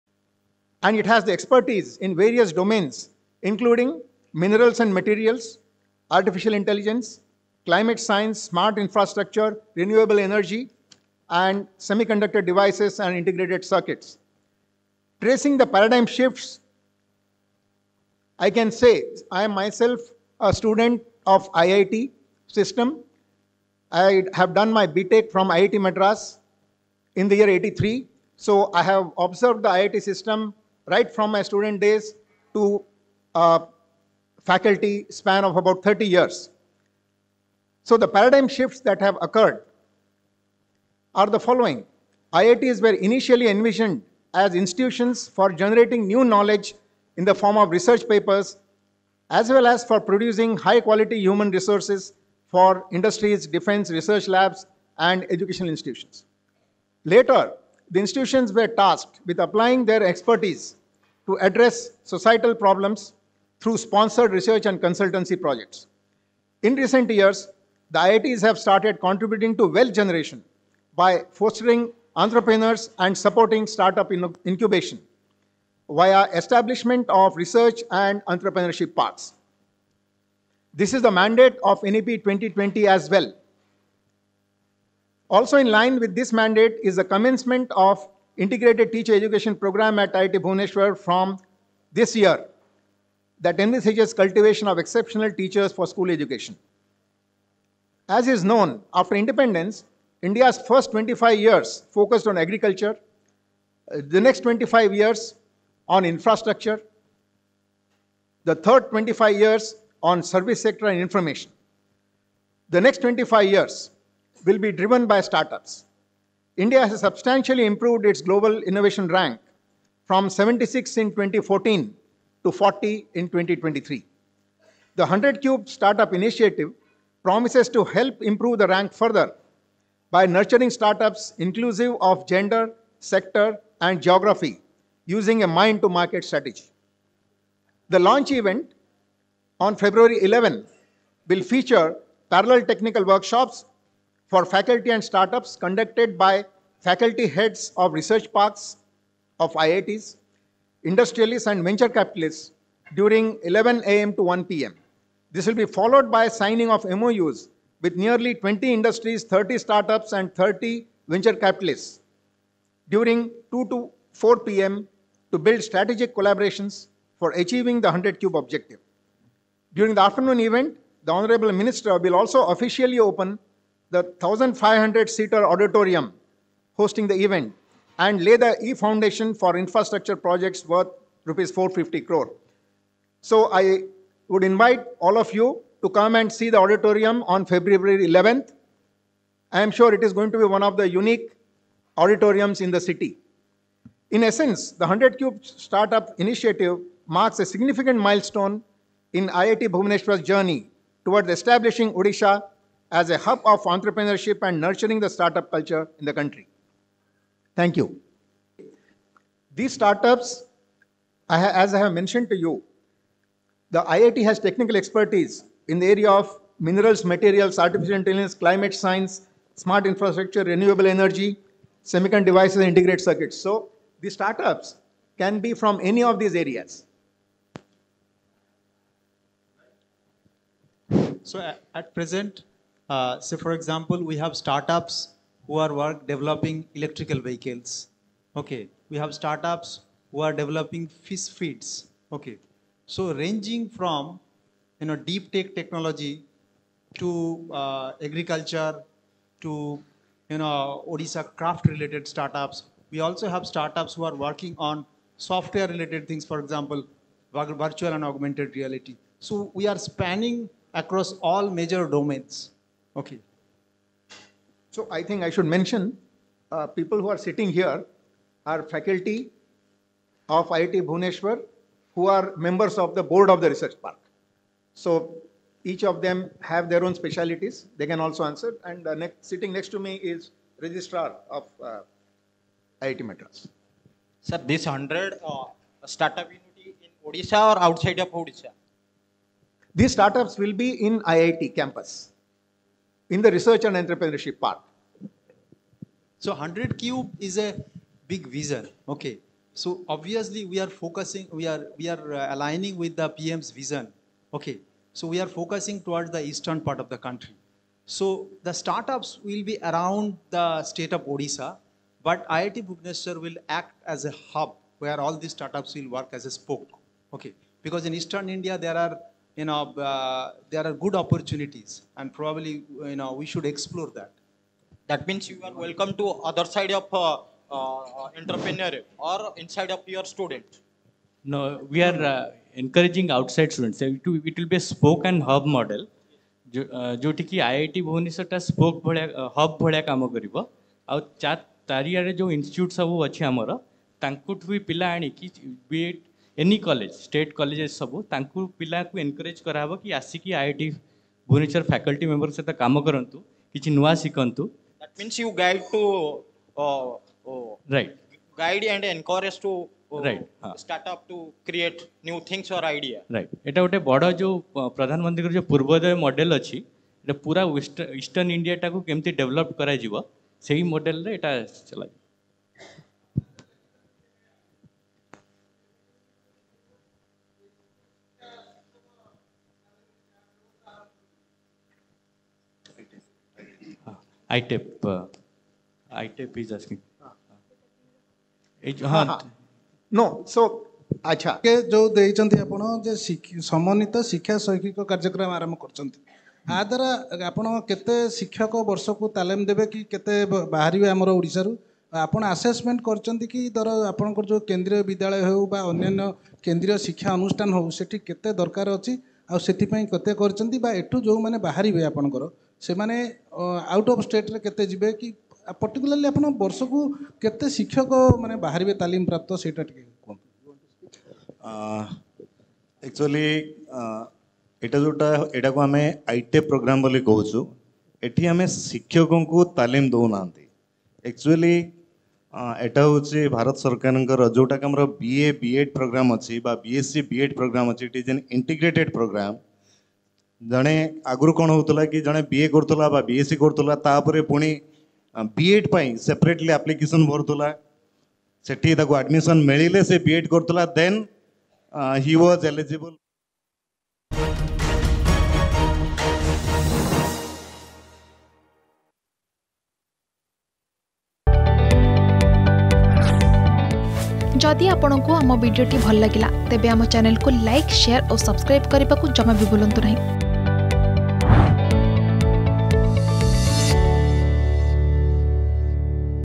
୧୦୦ କ୍ୟୁବ୍‌ ଷ୍ଟାର୍ଟ ଅପ୍‌ କନକ୍ଲେଭ || 100 Cube Startup Conclave
୧୦୦ କ୍ୟୁବ୍‌ ଷ୍ଟାର୍ଟ ଅପ୍‌ କନକ୍ଲେଭ । ଏଣ୍ଟରପ୍ରିନ୍ୟୁରସିପ୍‌କୁ ପ୍ରୋତ୍ସାହିତ କରିବାକୁ କାର୍ଯ୍ୟକ୍ରମ । ଦକ୍ଷତା ବିକାଶ ମନ୍ତ୍ରଣାଳୟ ପକ୍ଷରୁ କାର୍ଯ୍ୟକ୍ରମ ।